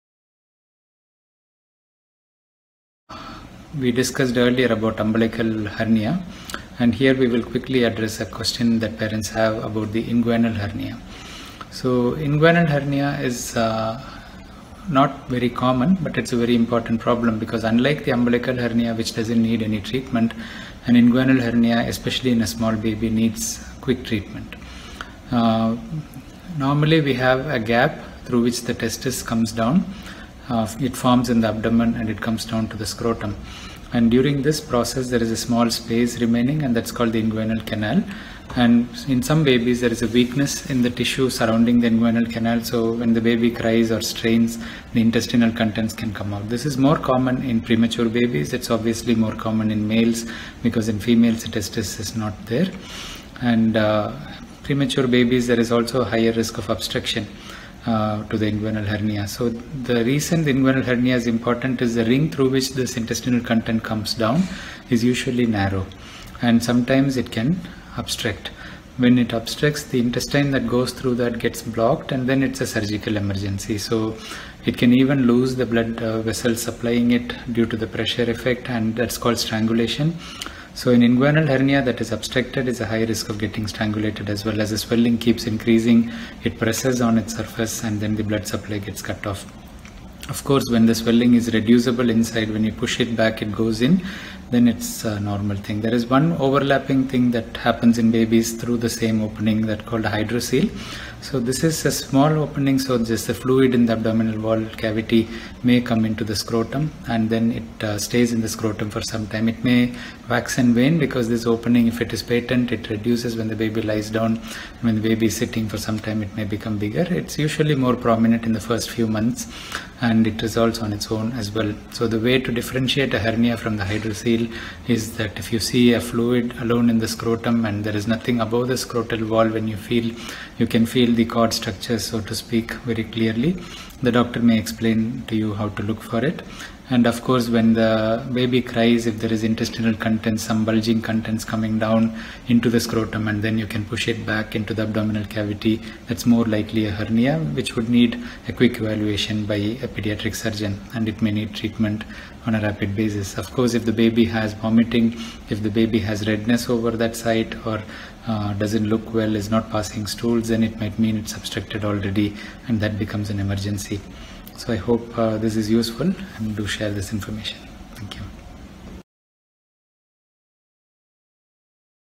What is an Inguinal hernia (in infants)? How do we treat it?
In this short video, I discuss what an inguinal hernia is and a quick overview of how to diagnose and treat the same.
Inguinal hernia is not very common but it is a very important problem. Unlike the umbilical hernia which doesn't need any treatment, an inguinal hernia, especially in a small baby, needs quick treatment. Normally, we have a gap through which the testis comes down, it forms in the abdomen and comes down to the scrotum. During this process, there is a small space
remaining, called the inguinal canal. In some babies, there is some weakness in the tissues surrounding the inguinal canal, and when the baby cries or strains, the intestinal contents can come out.
This is called inguinal hernia and is more common in premature babies. It is more common in males because in females, the testis is not there (and so no descent). In preterm babies, there is higher risk of obstruction to the inguinal hernia as well.
The reason why inguinal hernia is important is because the ring (gap) through which the intestines come out is usually narrow, and sometimes it can obstruct. When it obstructs, the intestine that passes through that gets blocked, and in that case, it is a surgical emergency. It can even lose the blood vessel supplying it due to the pressure effect and that's called strangulation.
An inguinal hernia that is obstructed is at high risk of strangulation as well. As the swelling keeps increasing, the blood supply gets cut off due to the pressure increasing and leads to strangulation. Of course, if the swelling goes back in on pressing, it is a non-obstructed  hernia.
One other thing that can pass through this opening is a hydrocele, which is due to a smaller opening and the fluid in the abdominal cavity may come into the scrotum and stays in the scrotum for some time . Its size can wax and wane because if this opening is patent, it reduces when the baby lies down and when the baby is sitting for sometime, it may become bigger. It is usually more prominent in the first few months and it resolves on its own as well. So the way to differentiate the hernia from the hydrocele is by feeling-if there is only fluid in the scrotum and nothing above (ie, we can feel the cord  structures above the scrotum), it is a hydrocele.
Your doctor may explain to you on how to look for it. Of course when the baby cries, if there is intestinal contents, you can see the bulging contents coming down into the scrotum and you can push it back into the abdominal cavity, it is likely a hernia and should need a quick evaluation by a pediatric surgeon. It may need treatment on a rapid basis.
Of course if the baby has vomiting, redness over the site of the swelling, doesn't look well or is not passing stools, it might mean it is obstructed already and is an emergency.
I hope this is useful, please do share.